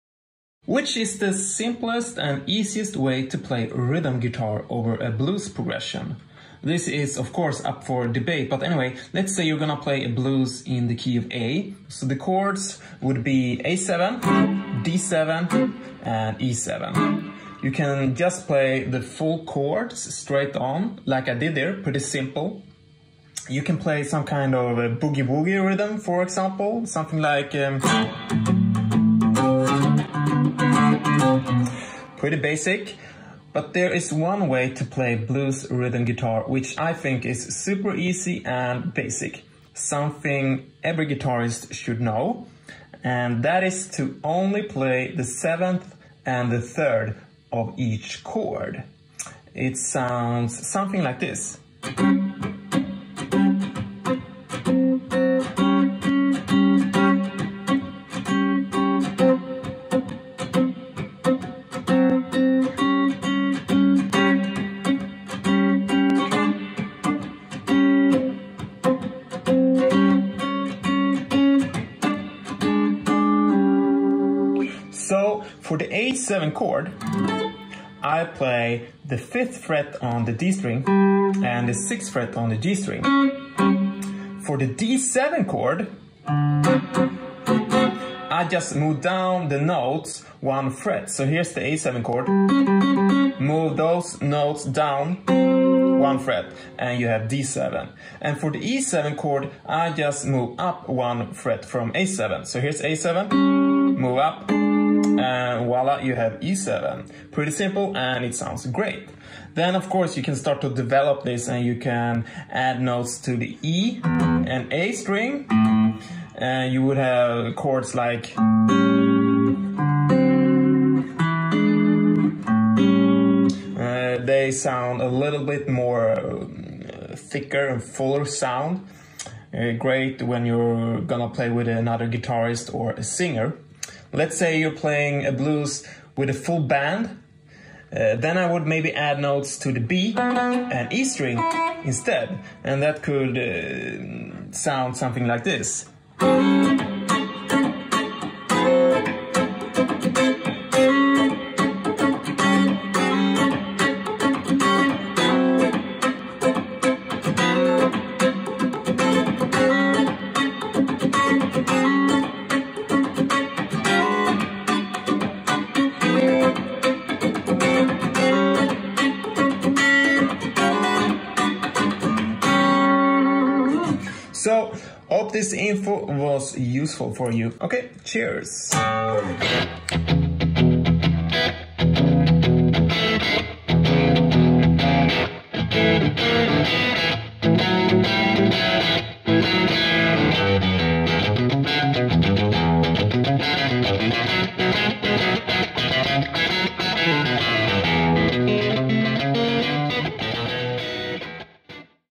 How To Play Blues Rhythm Guitar - Must Know Basics
In this short lesson I talk about Blues Rhythm Guitar and I show you what I think is the simplest and easiest way to play Rhythm Guitar over a Blues Progression. Really basic!
This is something every guitarist should know. From beginner to advanced,

I talk briefly about the chords you can play over a Blues Progression, in A Major.
I then show you something I think every guitarist should know - how to play Blues Rhythm Guitar using only the "minor seventh" and the "major third" from the chords. These "modified chords" can be referred to as "shell voicings".

I then show you how you can experiment and modify these "shell voicings" by adding more notes to the top two strings or adding the root notes on the E and A strings.